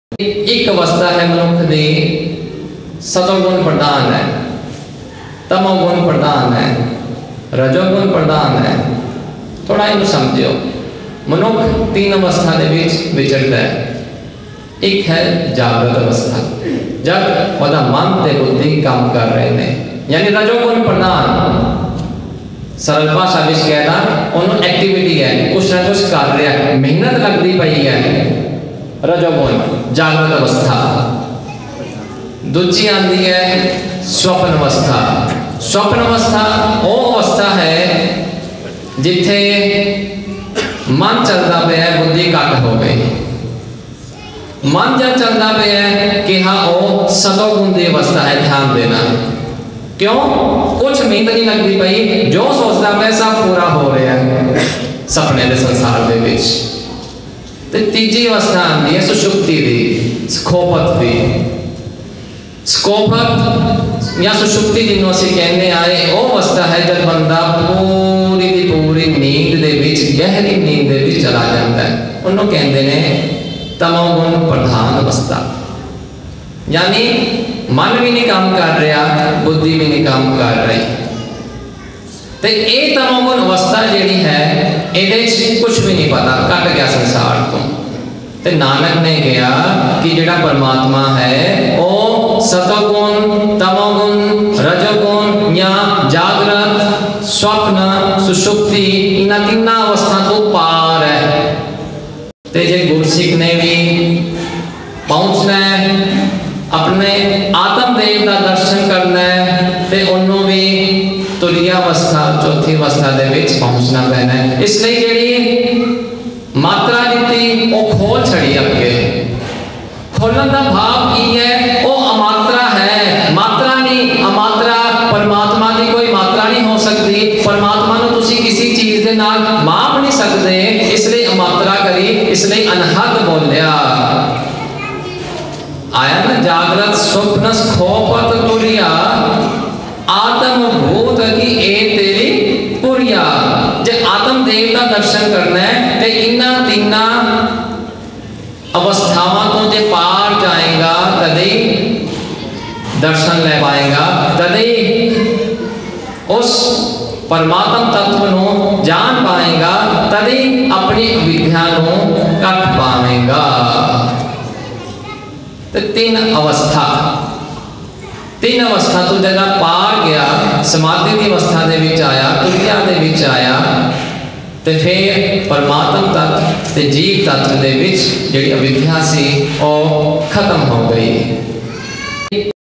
Individual Stages with its 3 mode of Material Nature
Jagrat Avastha : Waking state – when a person is fully aware of his body
Swapna Avastha : Dream State - a person remembers, those impressions which he/she gathers in the waking state, occur in the dreams.
Sushupti Avastha : Deep Sleep State –In that state  a person don’t know anything.
Swami Shree elucidates which guna is dominating in above stages. Lecture excerpted from Satsang held at Delhi-92 on occasion of Guru Nanak Jayanti, 2018.